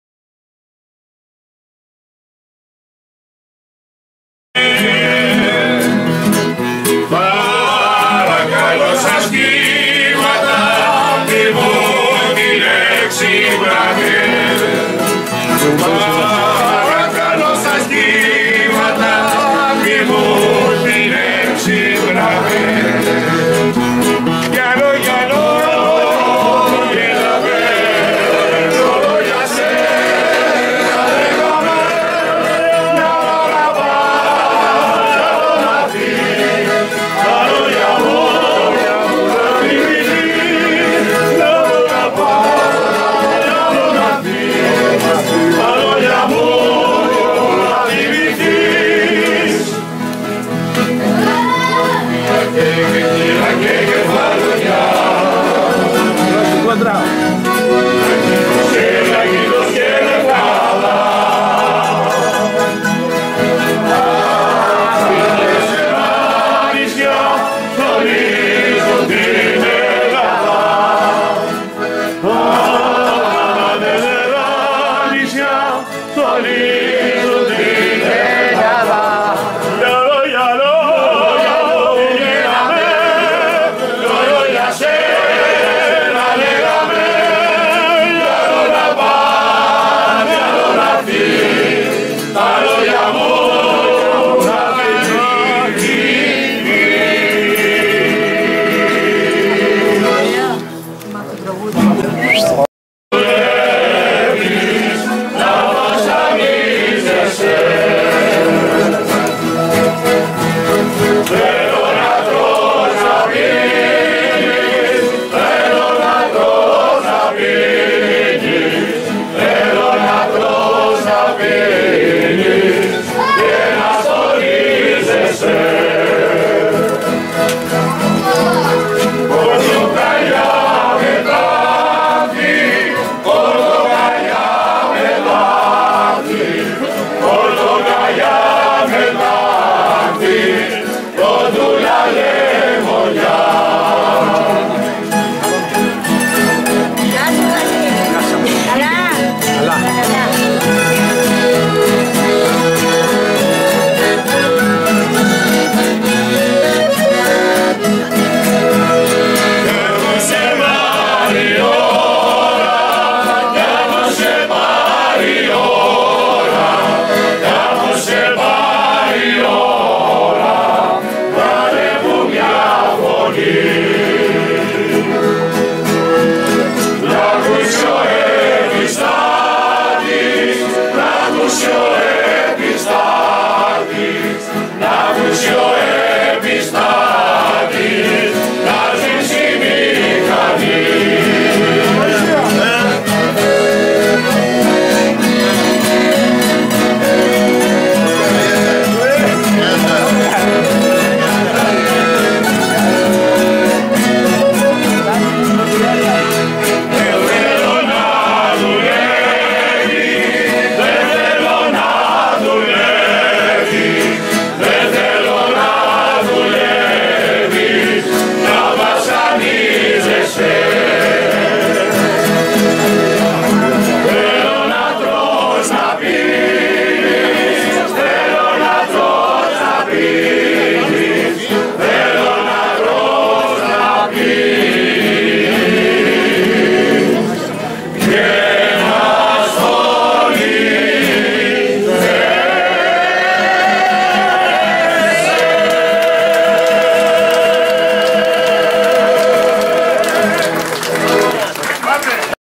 Βραδιά Καντάδας στο χωριό Άγιοι Δέκα, Κέρκυρας 2.
Από την εκδήλωση "Βραδιά Καντάδας" που διοργανώθηκε από τον Μορφωτικό Εκπολιτιστικό Σύλλογο Αγίων Δέκα την Δευτέρα 20 Αυγούστου 2012